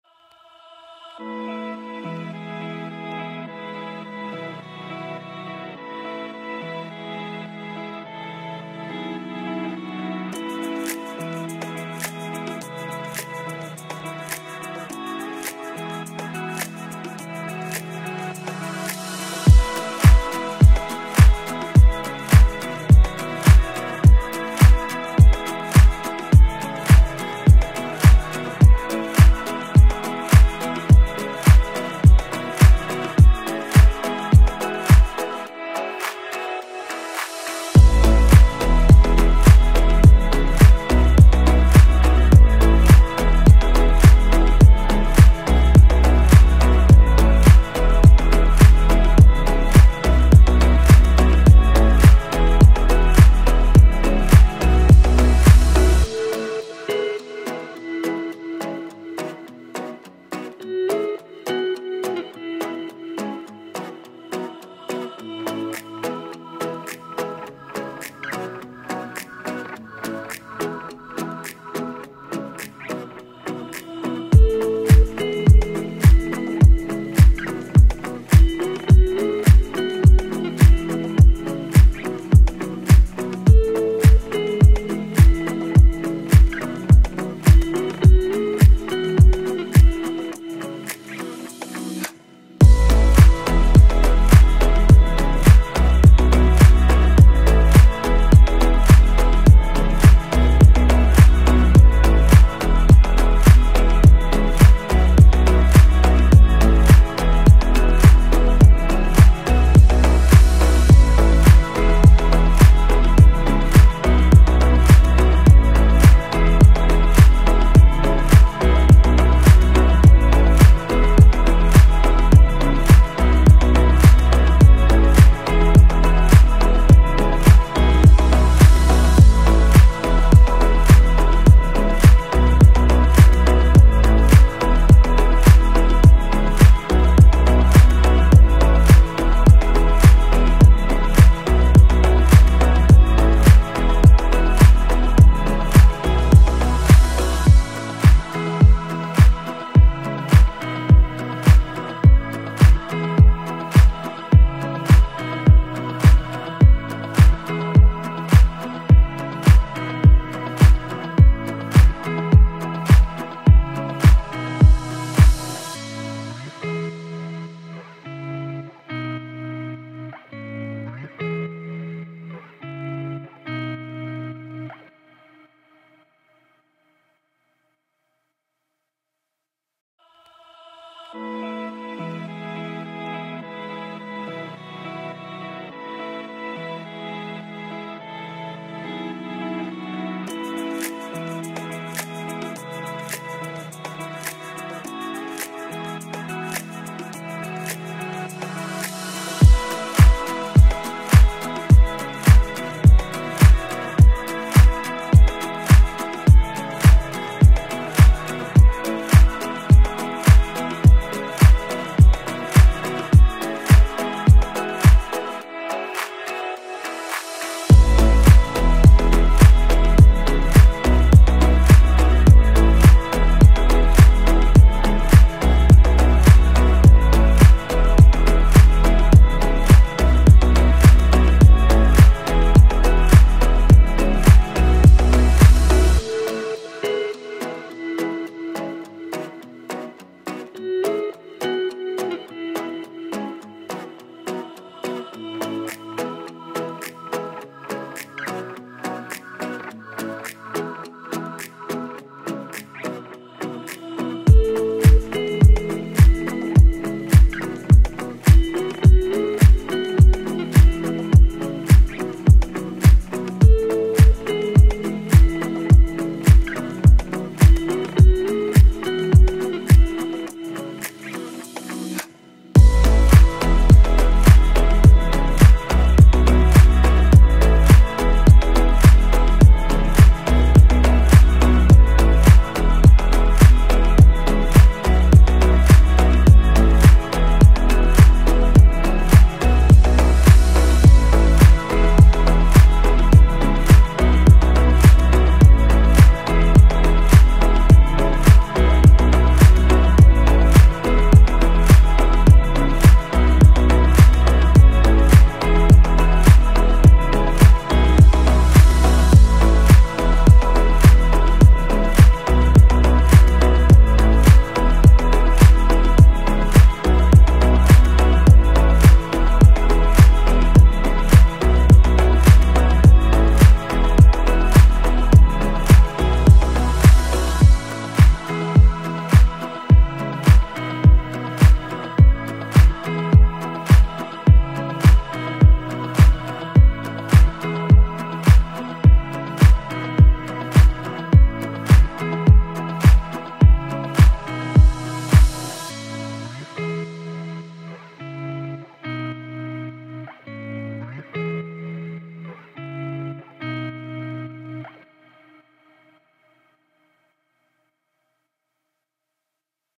GAC Art Buffet Instruction: Rug Hooking
Unleash your creativity with Gowld Art Center’s Art Buffet! This is the tutorial for rug hooking, an item to be chosen from our Art Buffet program, among a tutorial series to be posted on youtube! Follow our step-by-step tutorials and explore a variety of art techniques and mediums, from painting and craft to mixed media! Whether you’re a beginner or a seasoned artist, our Art Buffet has something for everyone. So grab the supplies we provide and let’s get creative!